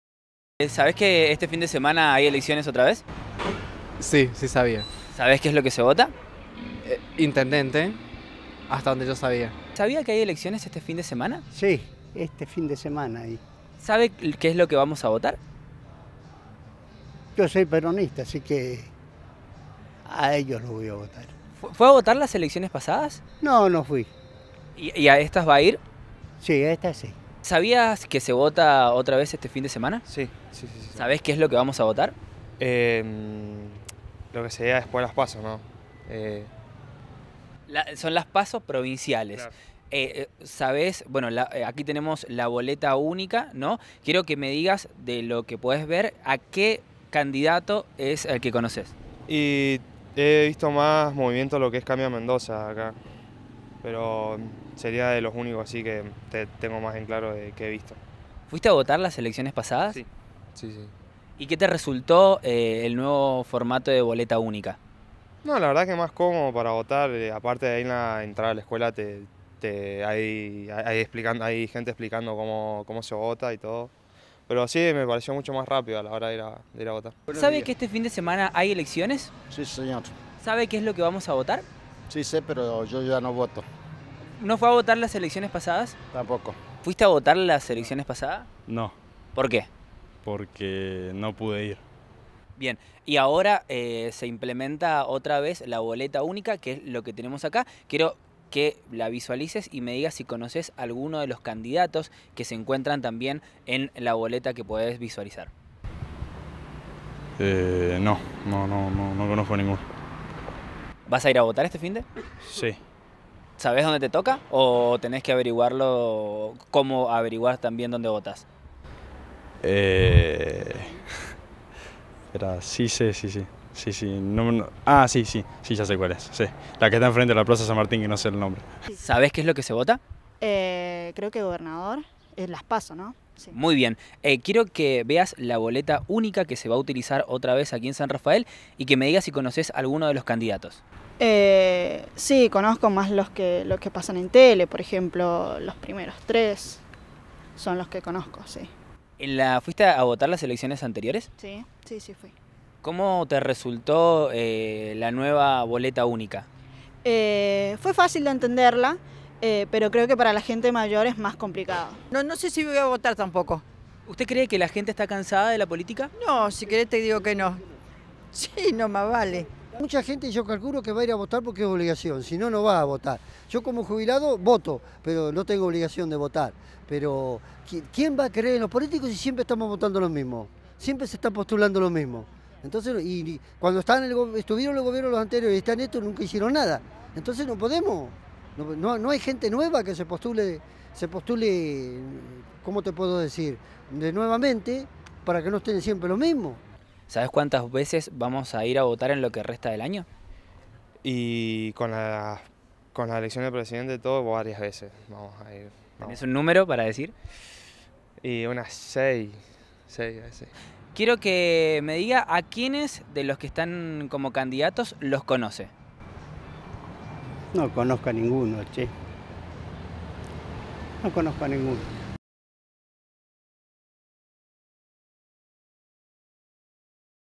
San Rafael - ¿Que saben los sanrafaelinos sobre las elecciones del 11 de junio?
El domingo 11 de junio, Los sanrafaelinos vuelven a ir a las urnas para votar. En este caso, se votará por candidatos a Gobernador, Vicegobernador y también Legisladores.
TVA salió a la calle a preguntarle a la gente qué sabe sobre estas elecciones P.A.S.O y qué piensan acerca de la cantidad de veces que hay que ir a votar en el año.